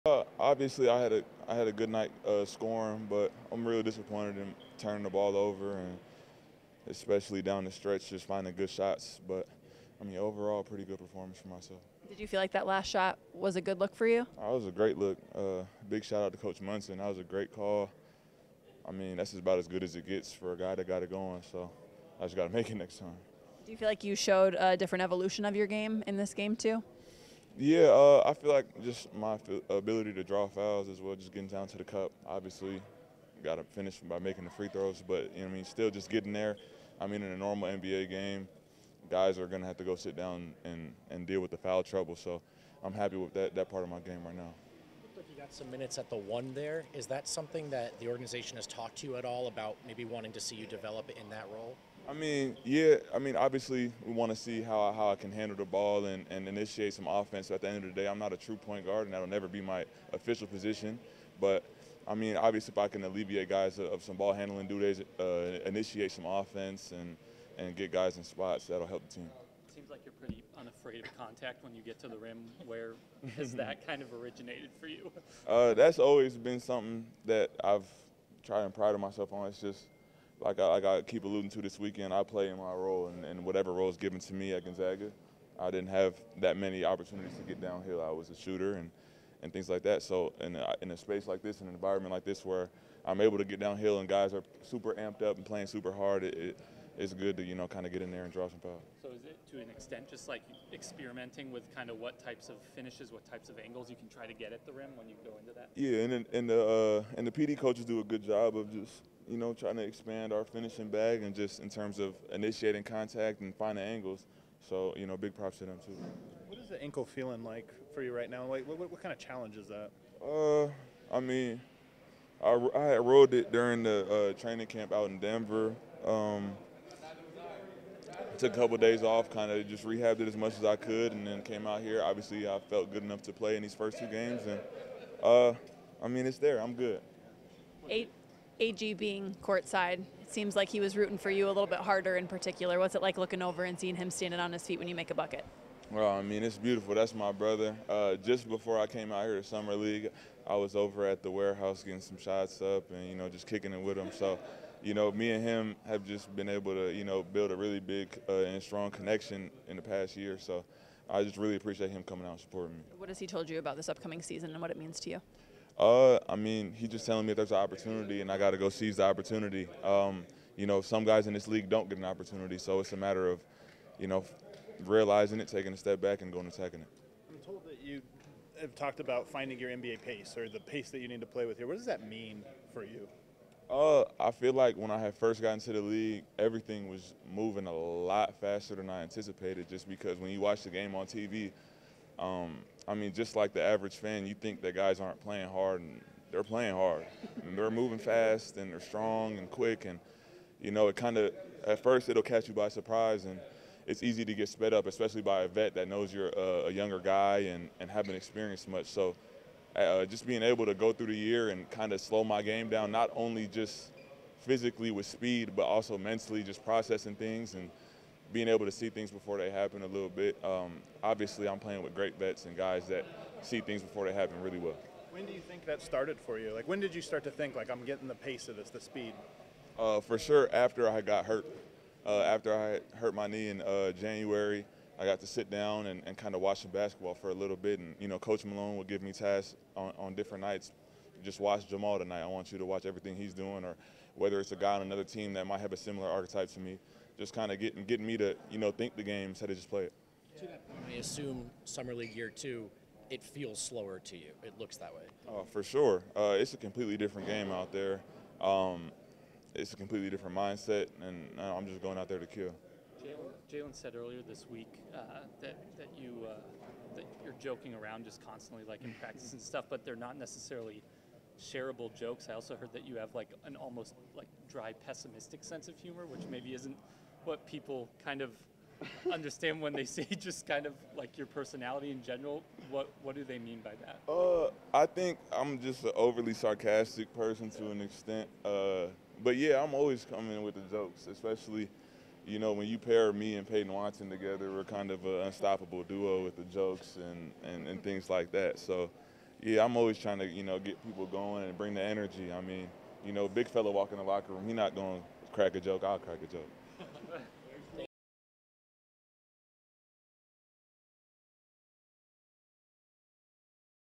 Julian Strawther Full Post Game Press Conference vs. Raptors 🎙 | NBA Summer League
Hear what Julian had to say after Game Two of NBA Summer League against the Raptors.